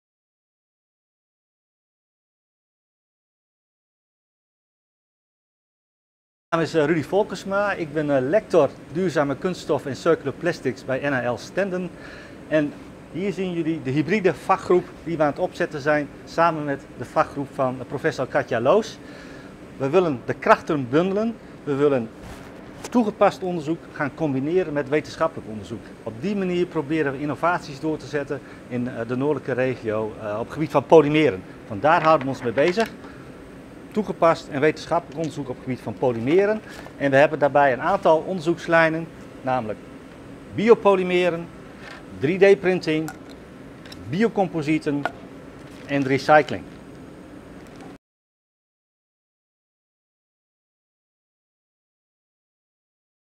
Meet the Green Chemistry Ecosystem of North Netherlands - 15 - Rudy Folkersma
Rudy is lector Sustainable and Circular Plastics at NHL Stenden. Together with the research department of professor Katja Loos, this is a hybrid research group, combining applied research the theoretical research. It involves four main research topics: bio-polymers, 3d-printing, bio-composites and recycling.